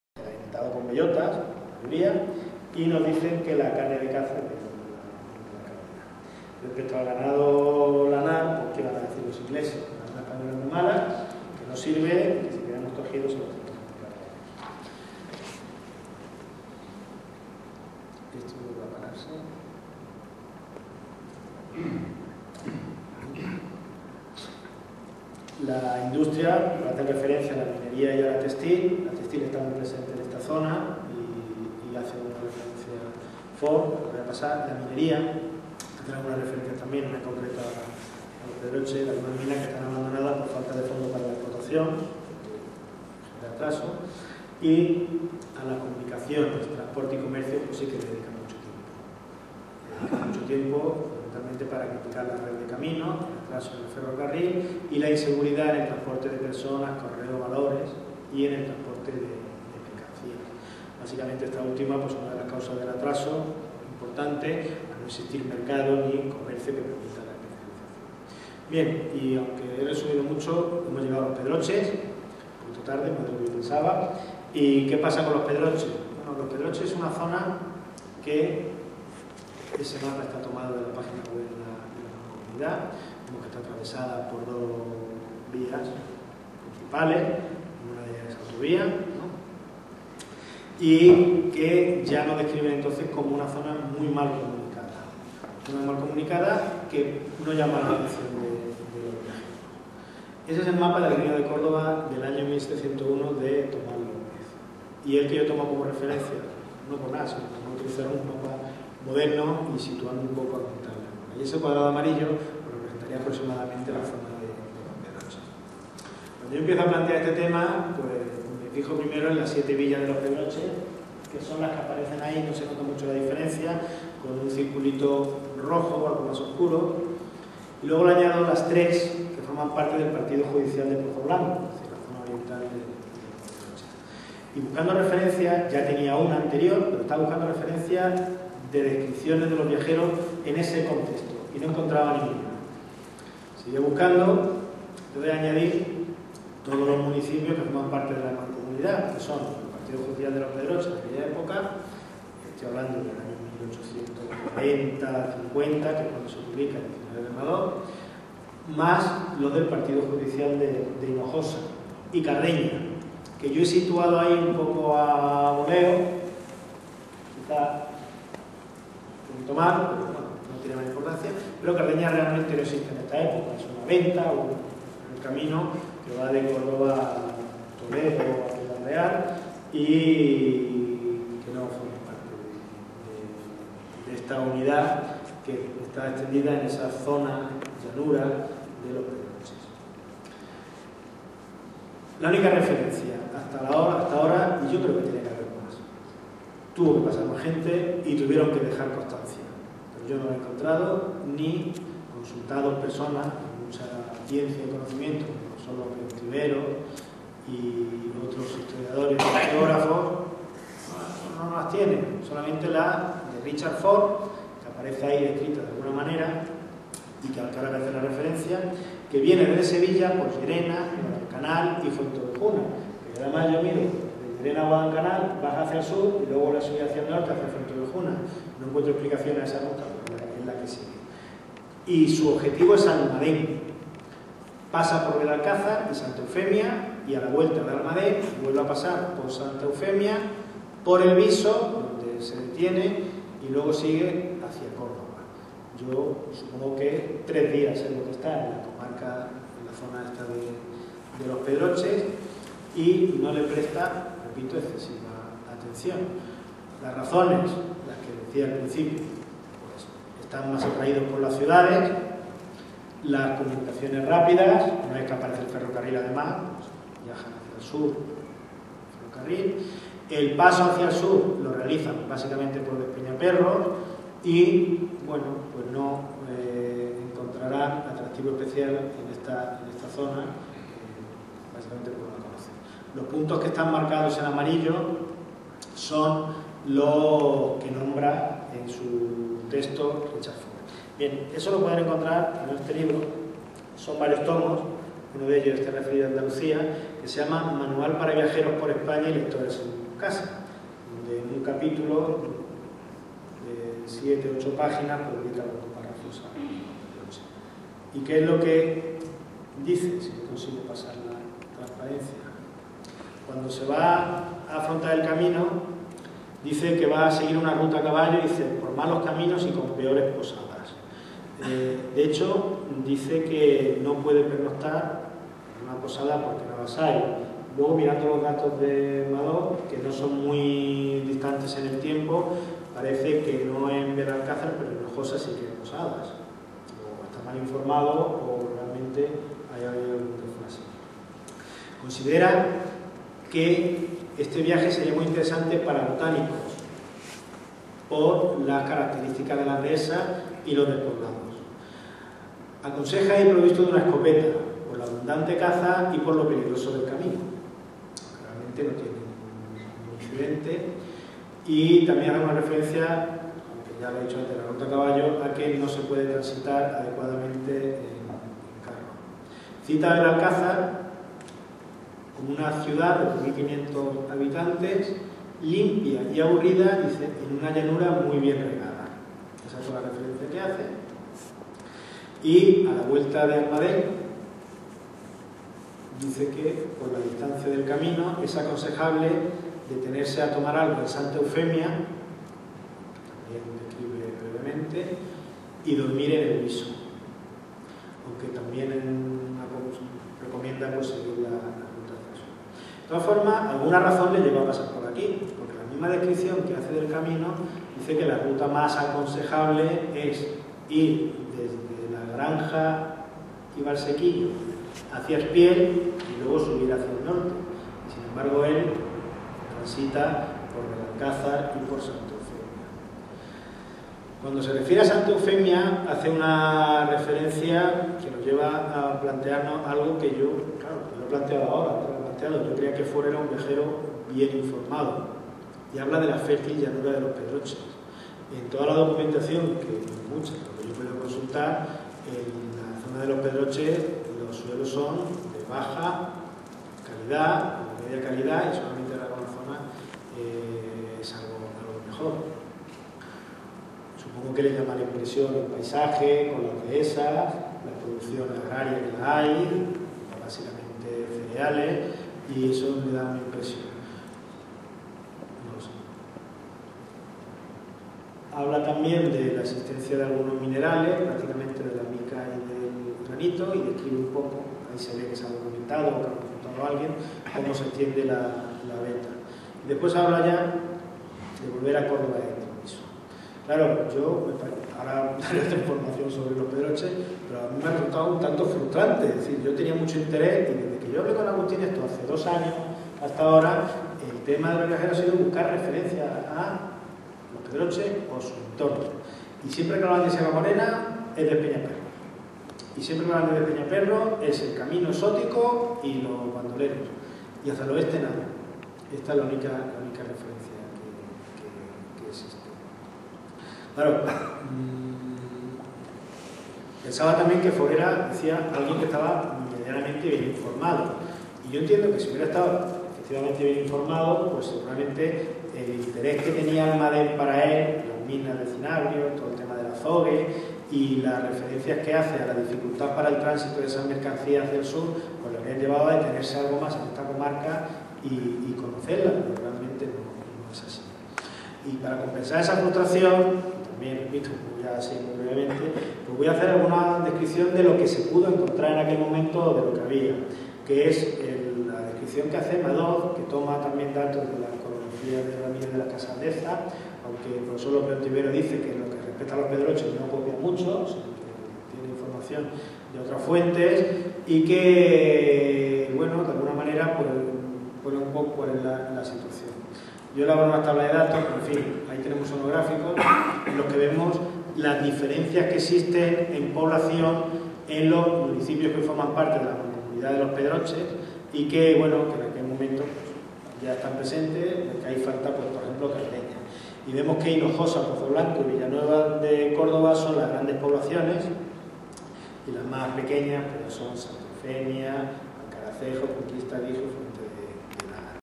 Viajeros Románticos en los Pedroches (VII). Dos Torres(Córdoba)
7 de agosto, segundo día de las XI JORNADAS DE HISTORIA Y DESARROLLO LOCAL.  En primer lugar la nuestra de Nuestro Tesoro del Archivo Municipal de Dos Torres, presentado por Agustín Galán. Y después la Conferencia: " La economía andaluza vista por los viajeros románticos del siglo XIX: una aproximación al Valle de los Pedroches" Impartida por Fernando Lara, Profesor de Economía Aplicada de la Faculta de Ciencias del Trabajo de la Universidad de Córdoba... Estas Jornadas están financiadas por UNIR, Universidad Internacional de la RiojaDía